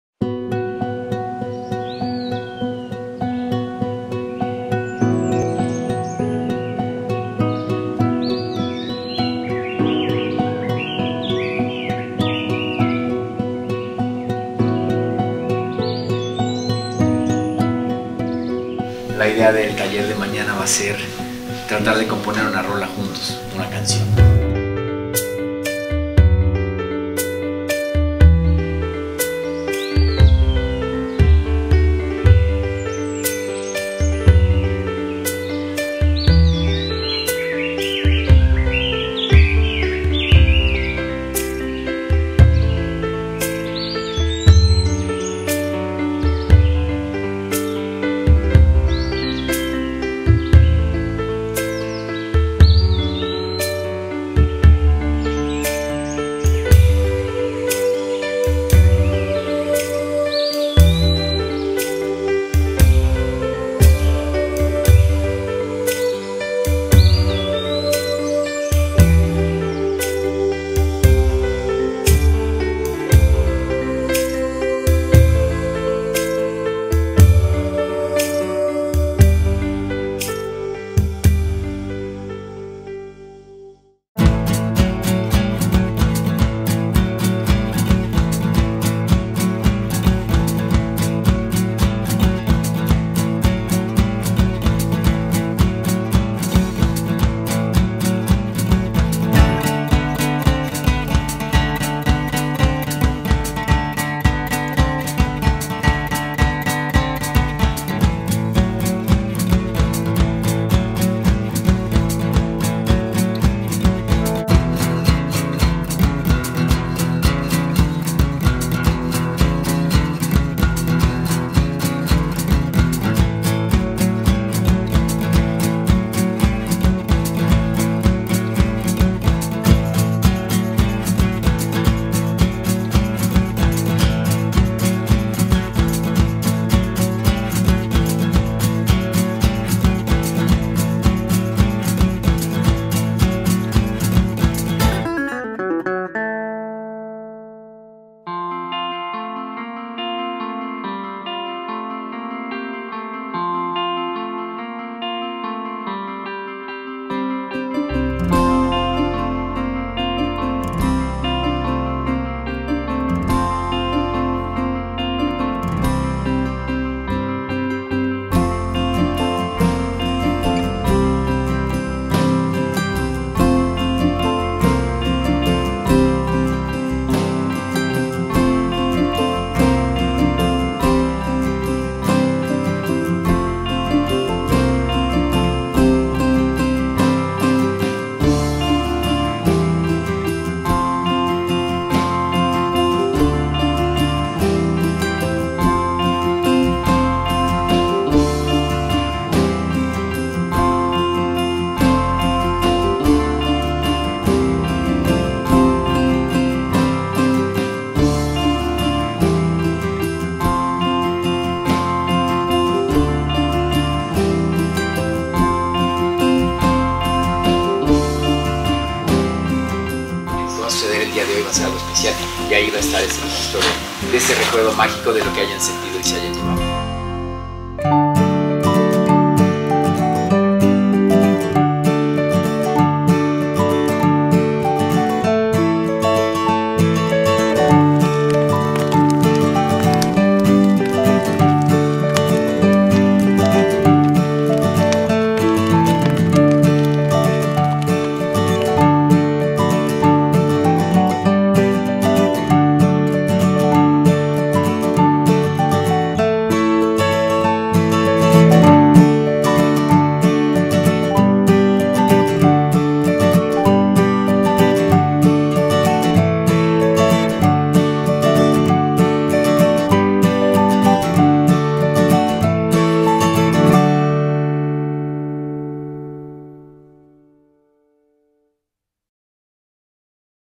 Taller de Creatividad noox impartido por Daniel Guiterrez vocalista de La Gusana Ciega
Del 18 al 20 de Enero de este año se llevó a cabo el Taller de Creatividad noox impartido por Daniel Guiterrez, vocalista de la Gusana Ciega. Los participantes compusieron y grabaron una canción en el estudio de música FRI, que se encuentra dentro del Rancho San Francisco, entre el increíble paisaje boscoso de las montañas hidalguenses.